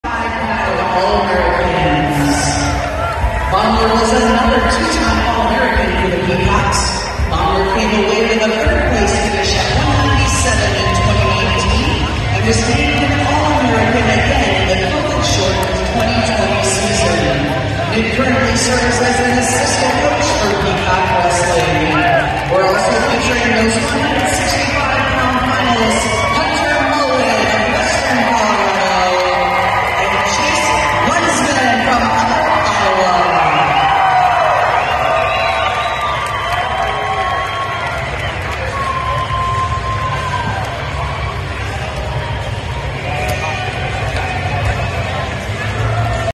All-Americans on parade
The 165-lb. All-American class for DII is brought out, including Chase Luensman of Upper Iowa, who qualified on Saturday morning for the finals.